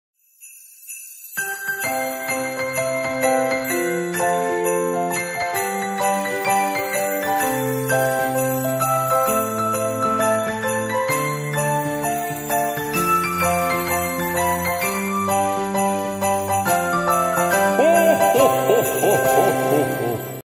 Abono fest (versión jingle)
Guerrero, ven a renovar o adquirir tu Abono Chiquito para el torneo Clausura 2016. ¡Te esperamos en el Abono Fest a partir de las 12 PM y hasta las 8 PM!